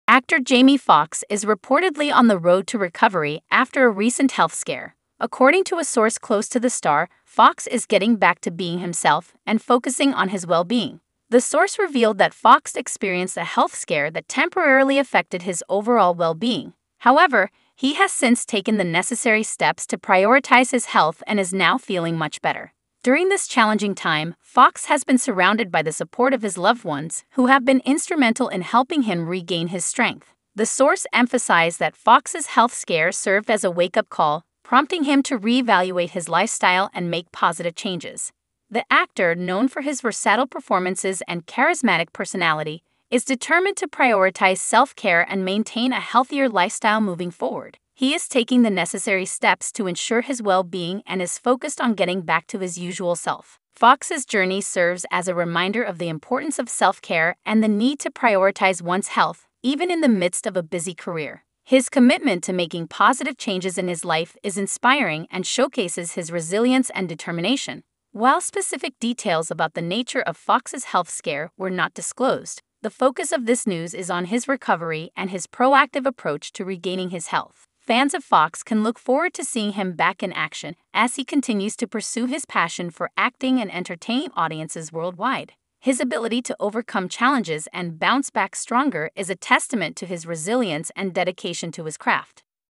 Jamie Foxx Is 'Getting Back to Being Himself' Following Health Scare, Source Says
Following a recent health scare, acclaimed actor Jamie Foxx is reportedly making significant progress in his recovery journey. According to a trusted source, Foxx is gradually "getting back to being himself" and focusing on rebuilding his health and well-being. The source shared that Foxx is taking the necessary steps to prioritize his physical and mental well-being, and is determined to regain his usual energy and vibrancy. Fans and supporters have expressed their relief and encouragement for Foxx during this challenging time, and are eagerly anticipating his return to the screen. Foxx's resilience and dedication to his health serve as an inspiration to many facing similar challenges.